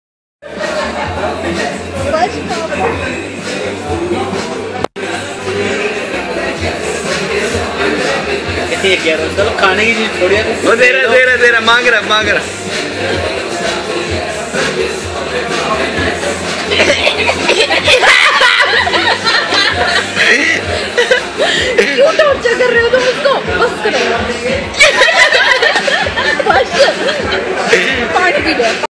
baby's reaction after having a lemon , epic :D
a small kid eating lemon and look at his reaction